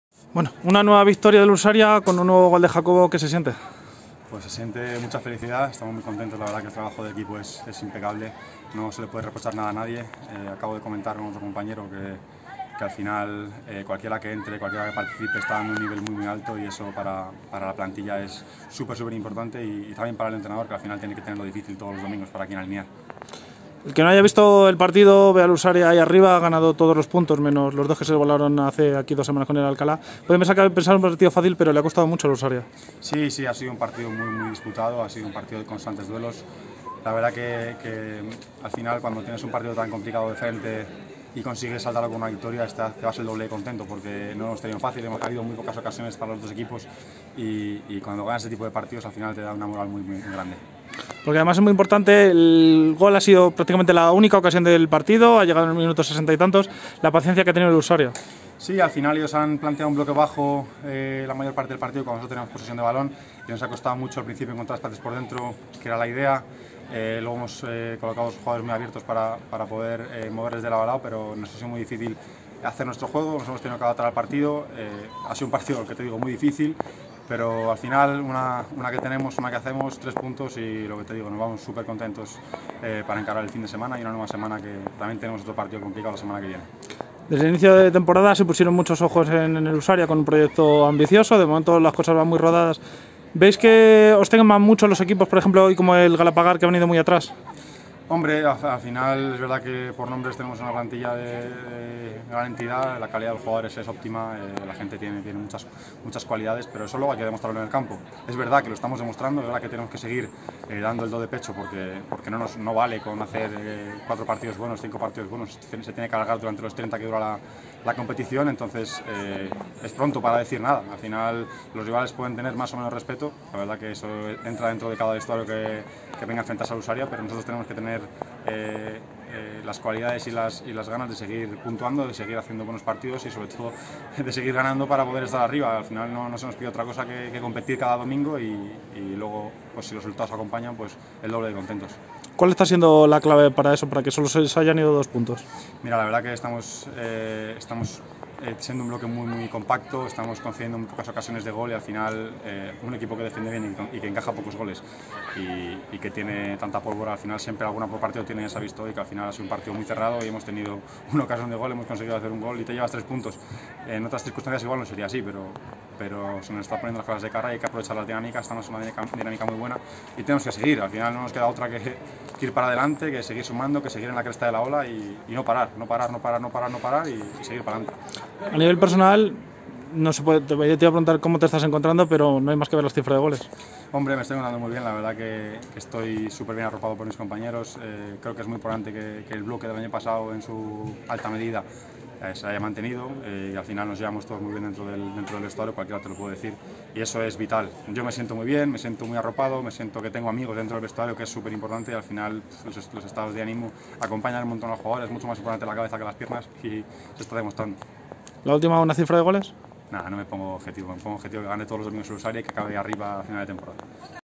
JACOBO ALCALDE, jugador del URSARIA: "No vale con hacer cinco partidos buenos"
Entrevista al jugador del Ursaria tras ganar al Galapagar